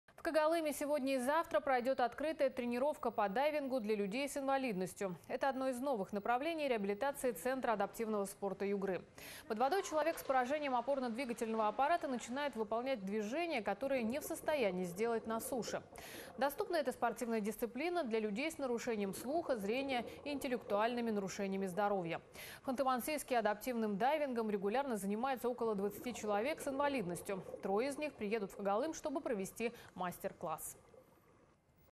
В Когалыме пройдёт мастер-класс по парадайвингу
В Когалыме сегодня и завтра пройдёт открытая тренировка по дайвингу для людей с инвалидностью. Это одно из новых направлений реабилитации Центра адаптивного спорта Югры.
Под водой человек с поражением опорно-двигательного аппарата начинает выполнять движения, которые не в состоянии сделать на суше. Доступна эта спортивная дисциплина для людей с нарушением слуха, зрения и интеллектуальными нарушениями здоровья.

По данным ОТРК «Югра», в Ханты-Мансийске адаптивным дайвингом регулярно занимается около 20 человек с инвалидностью. Трое из них приедут в Когалым, чтобы провести мастер-класс.